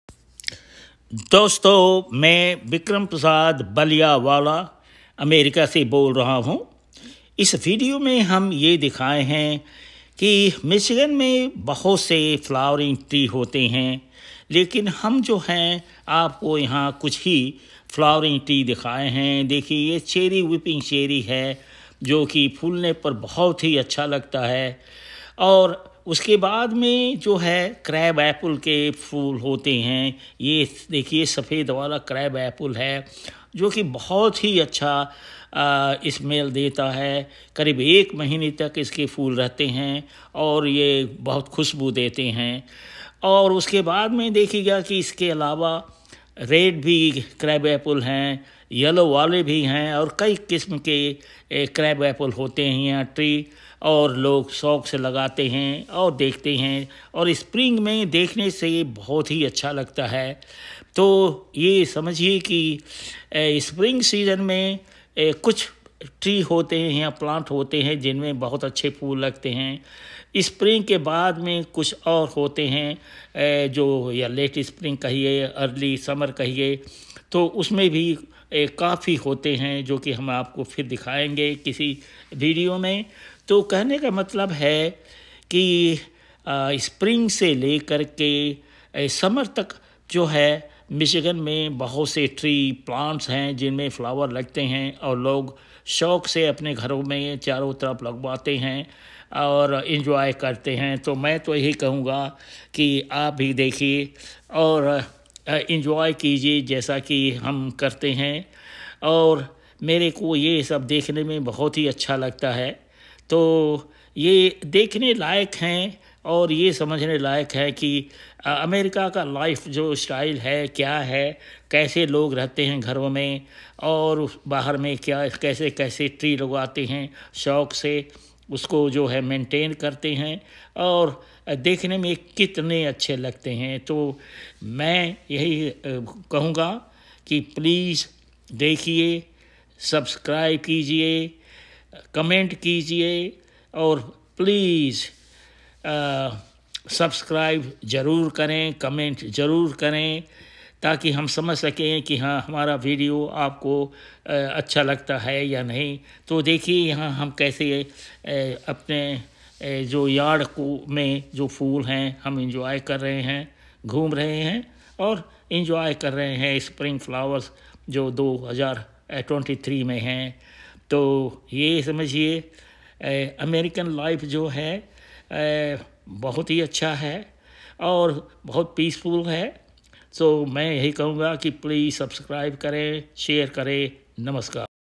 See some flowering trees of Spring season in 2023 [मिचिगन में कुछ बसंत मौसम के फुलो के पेड़ देखें]
State of Michigan is known to have many colorful flowering trees and plants in the Spring season. In this video, only some are shown that are present in the West Bloomfield Township [इस वीडियो में कुछ फूलो के पेड़ दिखाये हैं जिनके फूल देखने लायक़ हैं] floweringtrees michigan spring balliawala @vikramprasadballiawalinusa